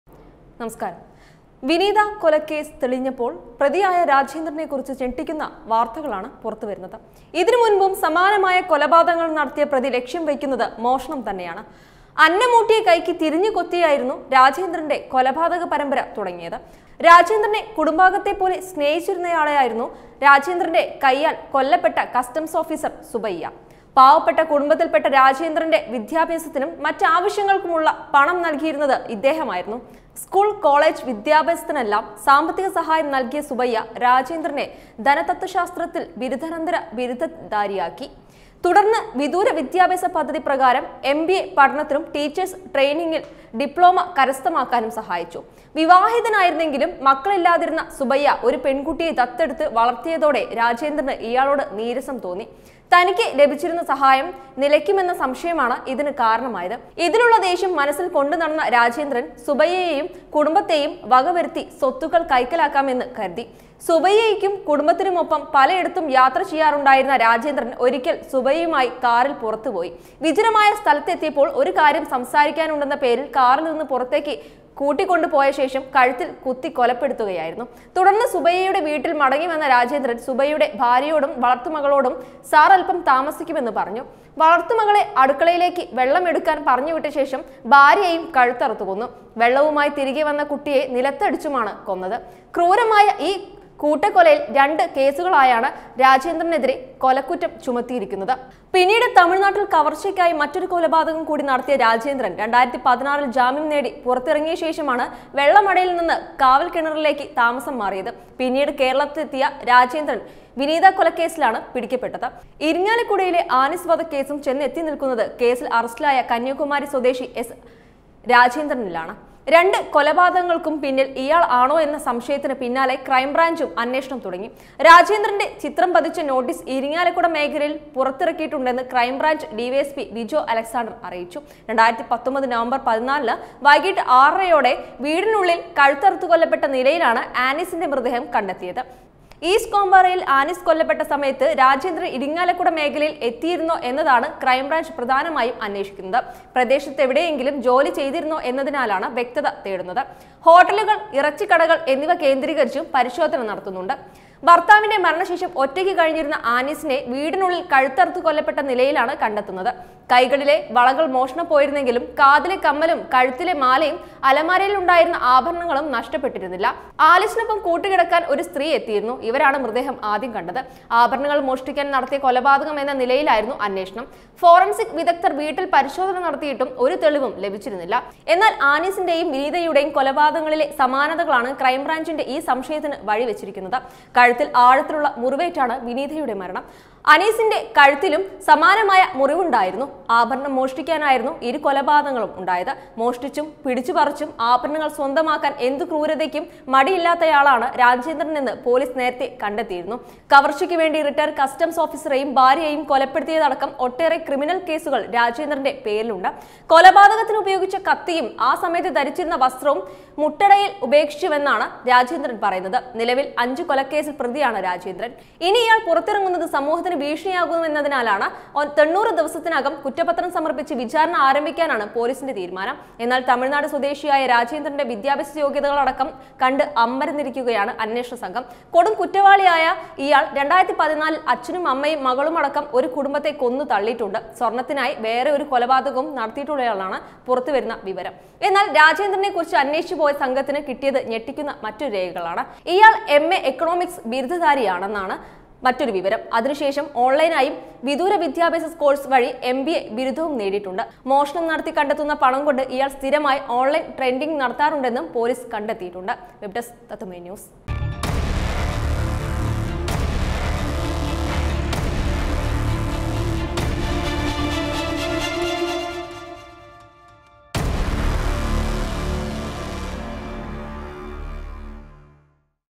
രാജേന്ദ്രന്റെ കൊലപാതക പരമ്പര ആരംഭിച്ചത് അന്നമൂട്ടിയ കൈക്ക് തിരിഞ്ഞു കൊത്തി
വിദ്യാസമ്പന്നനായ രാജേന്ദ്രൻ കൊലപാതകകഥകൾ കേട്ട് അറപ്പോടെ
പോലീസ് നേതൃത്വം